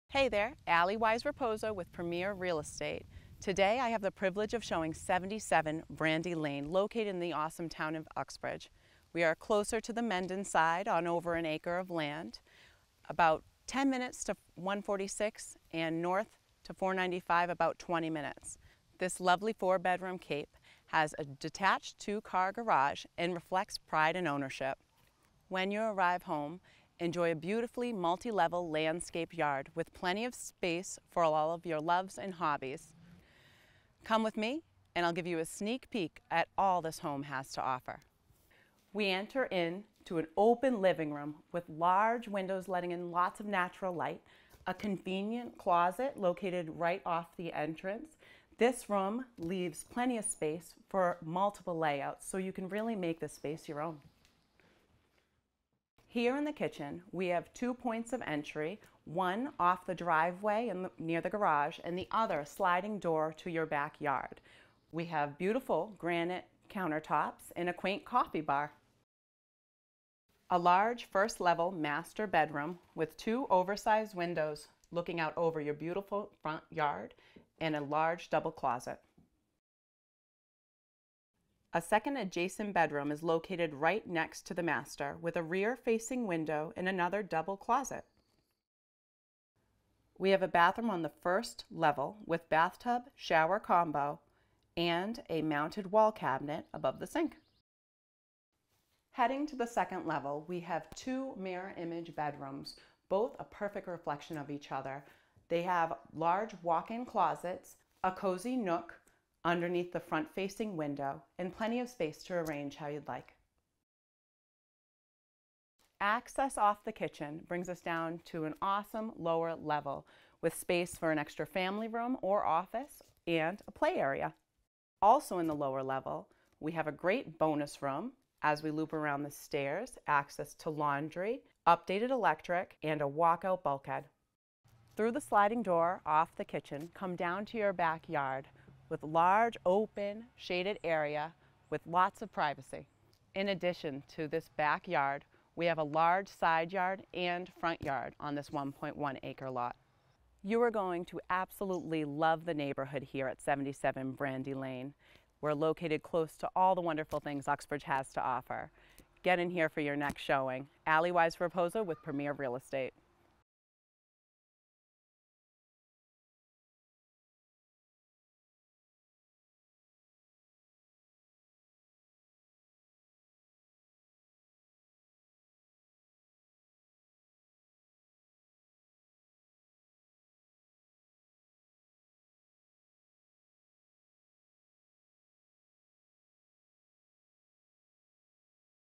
77 BRANDY LANE, UXBRIDGE, MA: Specious 4 Bed Cape on 1.01 Acres!
77 BRANDY LANE, UXBRIDGE, MA: Must See Location! Conveniently located on the Mendon side of Uxbridge, this beautifully landscaped Cape sits at the top of a stunning 1.01 acre lot in a highly sought-after neighborhood. Enter through the front to a main level large bright inviting living-room, with convenient entryway closet. This room opens to the kitchen, featuring a walkout sliding doors to a private deck, durable granite countertops with upgraded appliances & quaint breakfast nook. This level features the main bedroom and a additional bright 2nd bedroom toward the rear both allowing plenty of storage with with double closets. On the 2nd level you will find two more mirroring front to back bedrooms each with walk in closets. Additional living space on a lower-level has been finished with family room and bonus room being used as a gym. Outside enjoy a large two-car detached garage has plenty of upper storage, and shed. Vinyl siding, younger roof, and a passed title V makes this the perfect Move in Ready home.